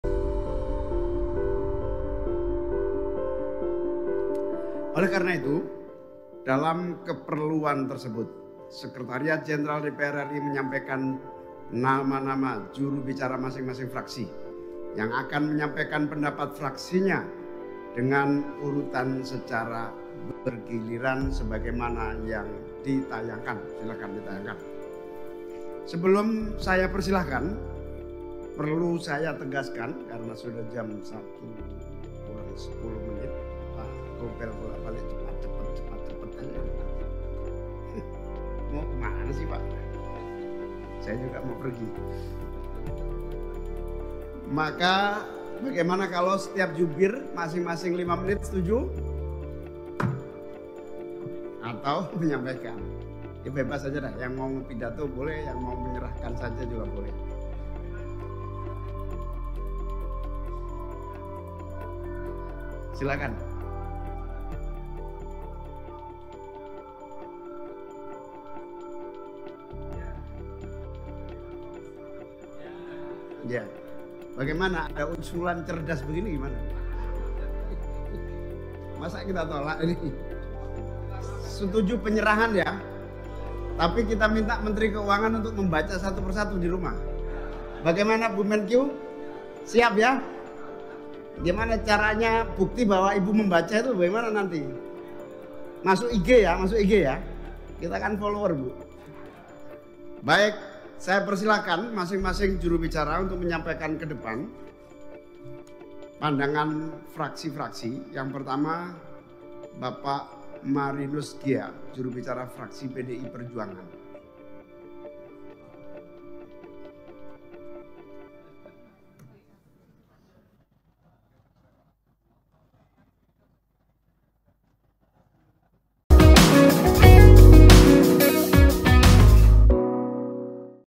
Sidang Paripurna DPR Dipimpin Muhaimin Iskandar, Sri Mulyani Diminta 'Unggah Foto' di IG
Wakil Ketua DPR RI Muhaimin Iskandar memimpin Sidang Paripurna penyampaian pandangan fraksi-fraksi atas RUU.

Sidang Paripurna ke-21 Masa Persidangan V ini dilaksanakan di Gedung Nusantara II, Senayan, pada Selasa (09/7/2024).

Dalam rapat paripurna tersebut, hadir juga Menteri Keuangan (Menkeu) Sri Mulyani Indrawati.

Namun, karena keterbatasan waktu, Muhaimin menawarkan agar masing-masing fraksi menyampaikan pandangannya selama 5 menit.

Momen lucu terjadi saat Menteri Sri Mulyani berjanji akan memfoto dirinya membaca pandangan fraksi tersebut.

Cak Imin pun menanggapi jawaban Sri Mulyani dengan gestur dari jauh, sambil berkata, "Nanti masuk IG ya Bu!"
Video terkini dan terbaru..